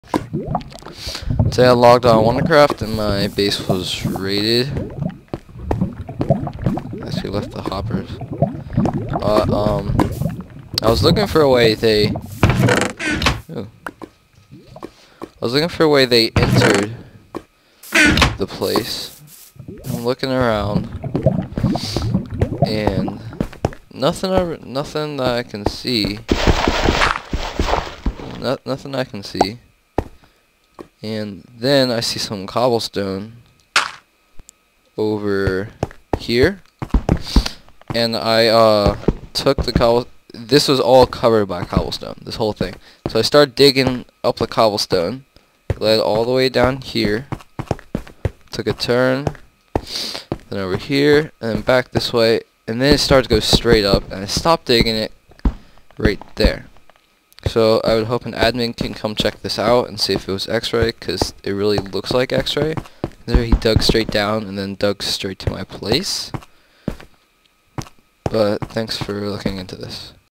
WanderCraft Base X-Rayed... Again
This shows whar I believe to be xray and if a Mod/Admin could check into this, Id appreciate it.